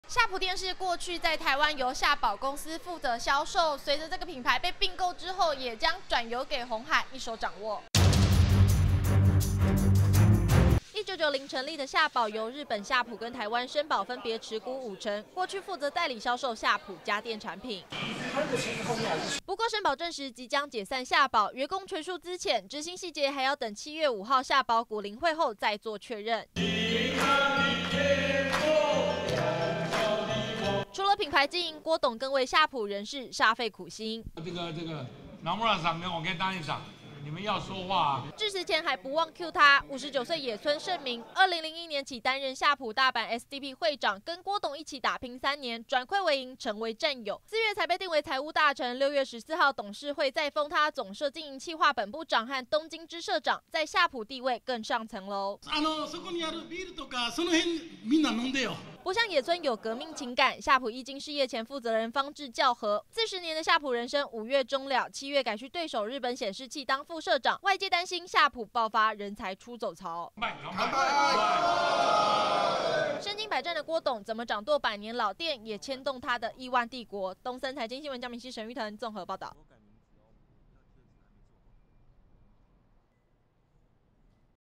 夏寶將熄燈?!鴻海奪回夏普品牌經營
郭董入主夏普後，再度出招！過去夏普電視，靠聲寶旗下的夏寶負責在台銷售業務，現在鴻海要把品牌經營權重新拿回，聲寶也證實，夏寶公司員工將全數解散。